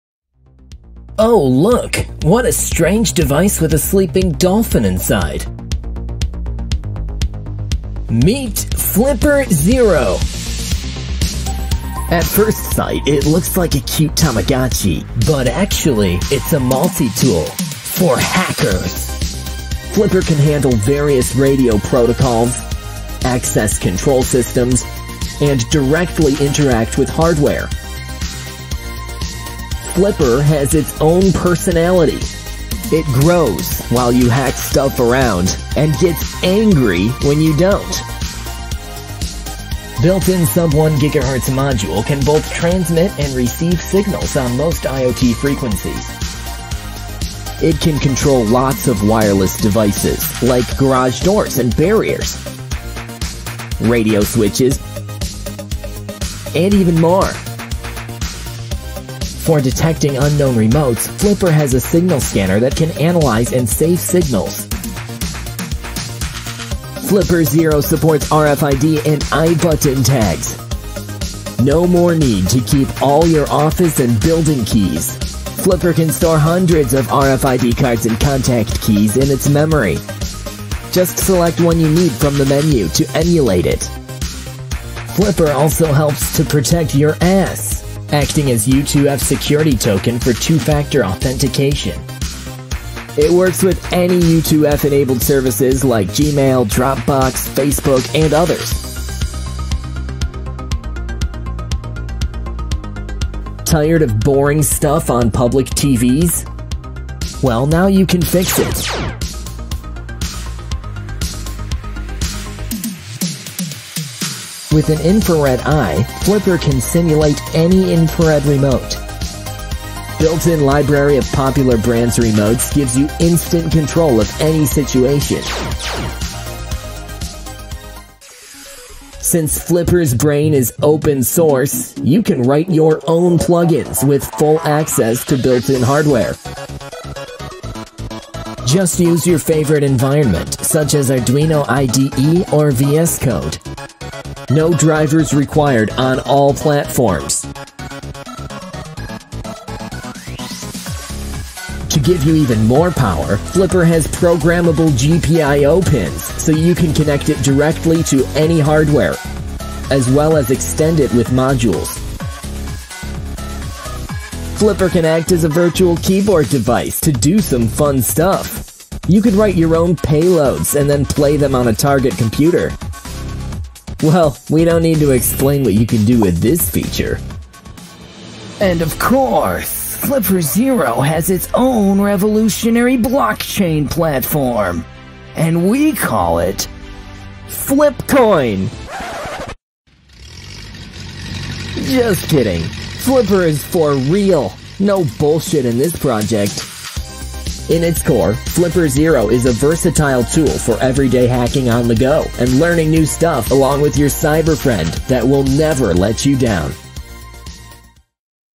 Flipper Zero — Tamagochi for Hackers
Open source multi-tool device for researching and pentesting radio protocols, access control systems, hardware, and more.